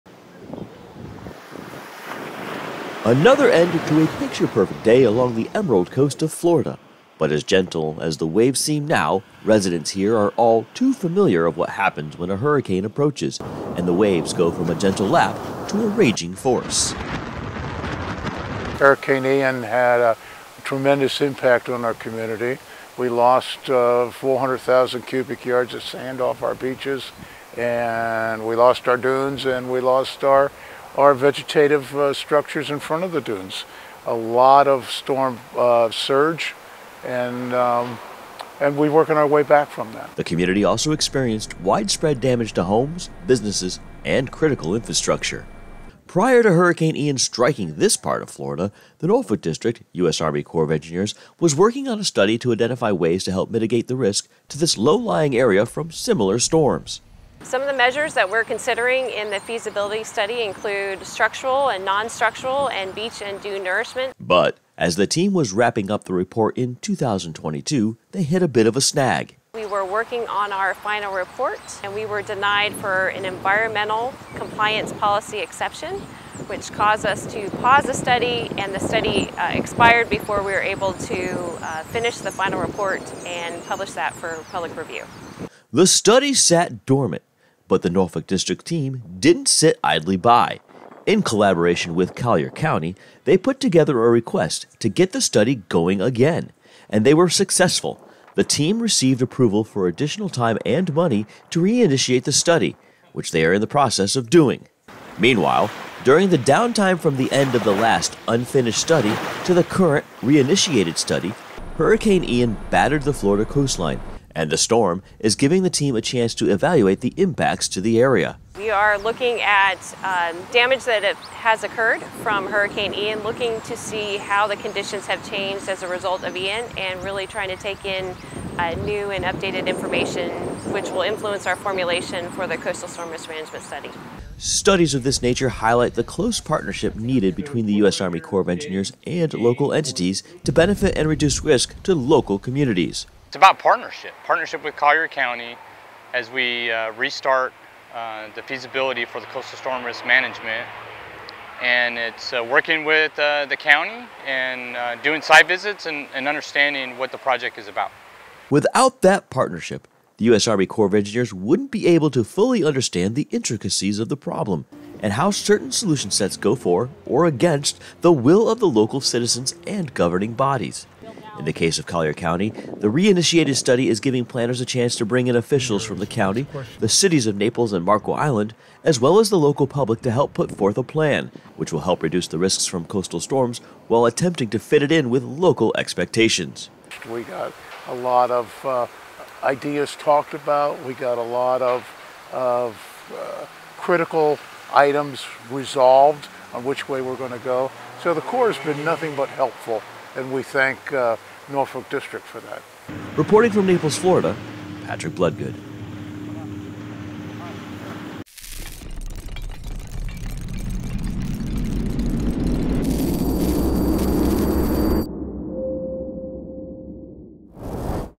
USACE Works to Re initiate Collier County CSRM
A small team consisting of leaders, planners, project managers, environmental scientist from the North Atlantic Division, Norfolk and Jacksonville districts, travelled around with representatives from Collier County, Florida looking at areas impacted by the effects of Hurricane Ian.

The team is working with county officials to reinitiate the Collier County Storm Risk Management feasibility study, which was nearing a final report in 2022 when a set back was realized.